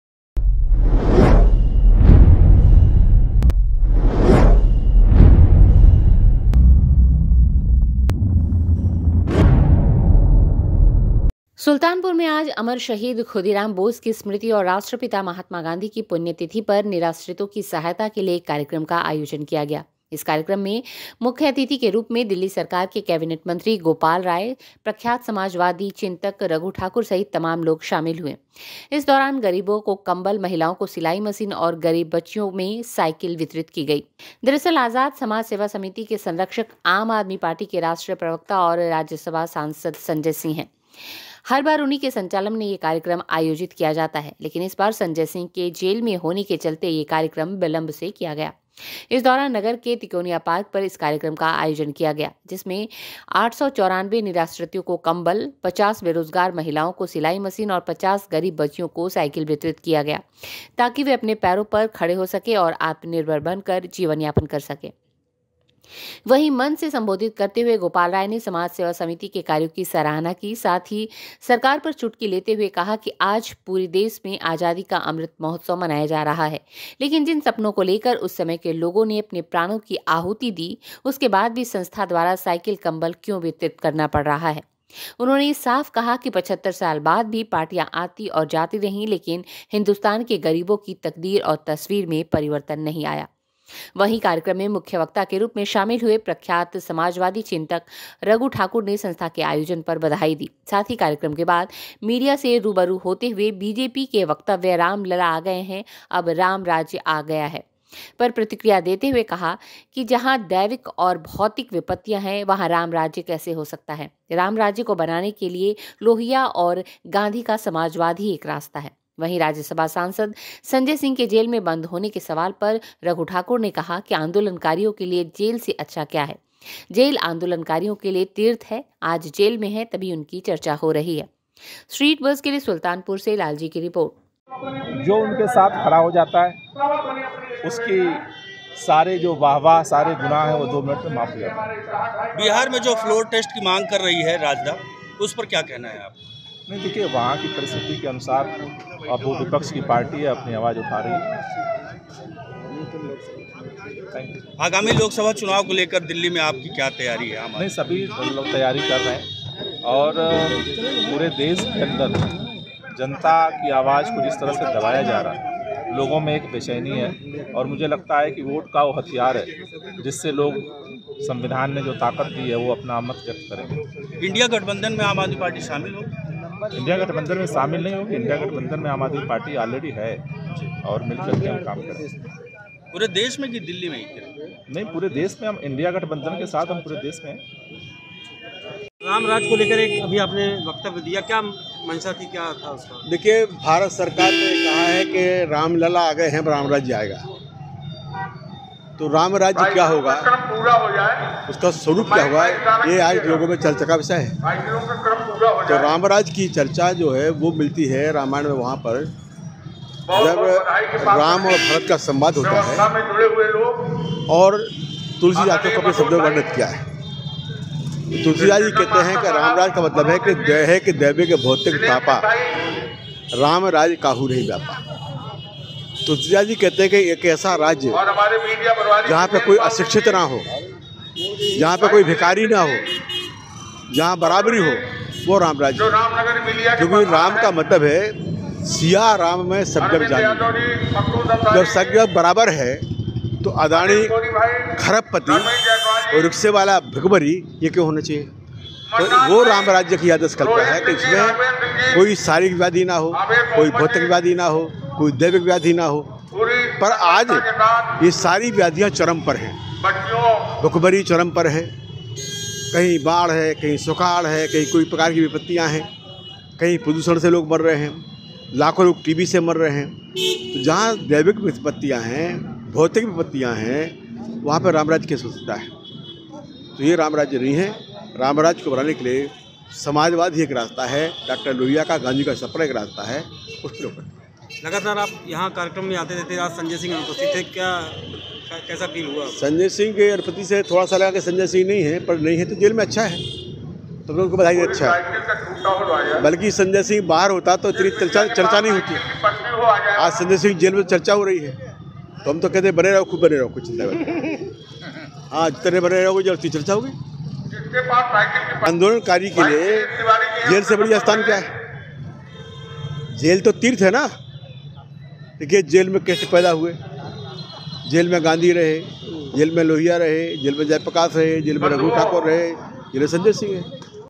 महिलाओं को सिलाई मशीन और गरीब बच्चियों को सायकिल वितरित किया गया
सुल्तानपुर में आज अमर शहीद खुदीराम बोस की स्मृति और राष्ट्रपिता महात्मा गांधी की पुण्यतिथि पर निराश्रितों के सहायतार्थ एक कार्यक्रम का आयोजन किया गया। इस कार्यक्रम में मुख्य अतिथि के रूप में दिल्ली सरकार के कैबिनेट मंत्री गोपाल राय, प्रख्यात समाजवादी चिंतक रघु ठाकुर सहित तमाम लोग शामिल हुये। इस दौरान गरीबों को कम्बल, महिलाओं को सिलाई मशीन और गरीब बच्चियों को सायकिल वितरित किया गया।